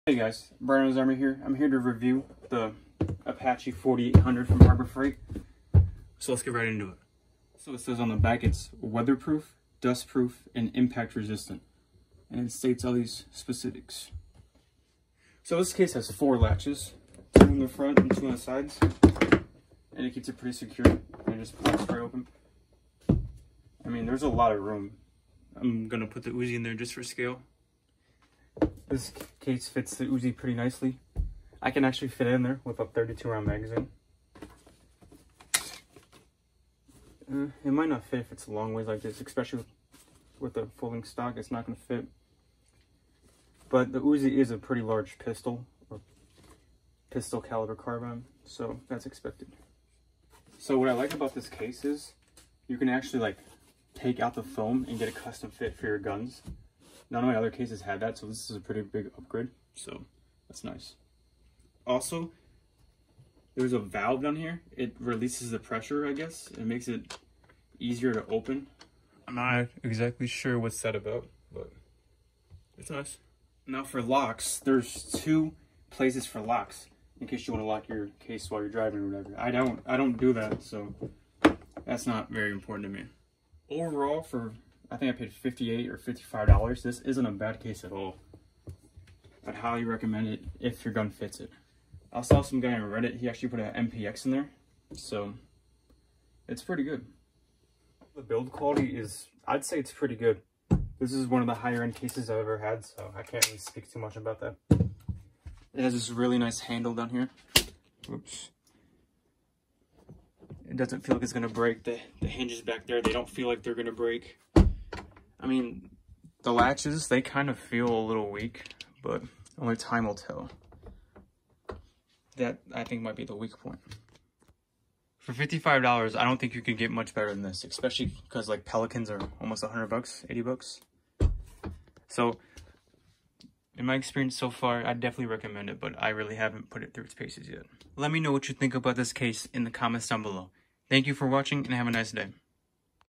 Harbor Freight Apache 4800 Gun Case Review!
Hey guys! In this video I show my thoughts on the harbor freight Apache 4800 gun case.